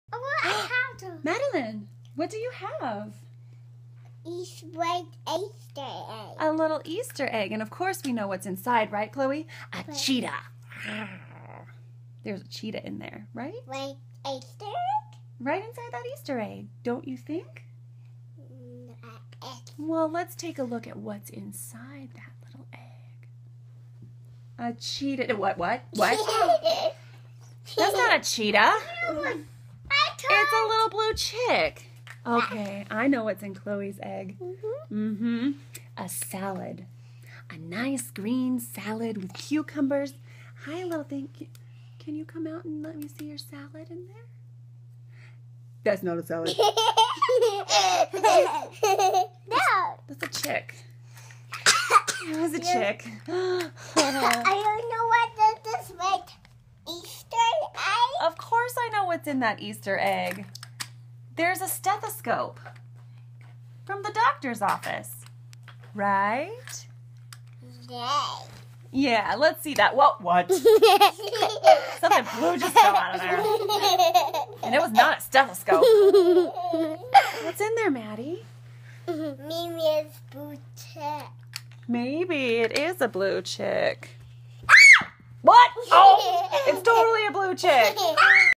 What Is In There?! A Toddler Guessing Game (ages 1-3)
Not only do these chicks promote good, silly fun... the learning is endless! Keri Poppins uses them in practicing all these skills too:
Color Identification/matching
Shape identification/matching
Sound imitation
Counting
Facial Expression Recognition
Fine Motor Working
Parallel Play
Guessing
Problem Solving
And MORE!

Super fun toy!